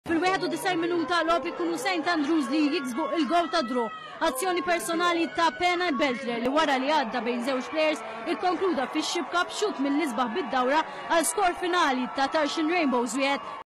Enmy Beltre Anota Golazo con el St Andrews FC!!!
El Dominicano y Seleccion Nacional Enmy Peña beltre Anota Otro Golazo en la Premier League Martensa Con el St Andrews FC!!!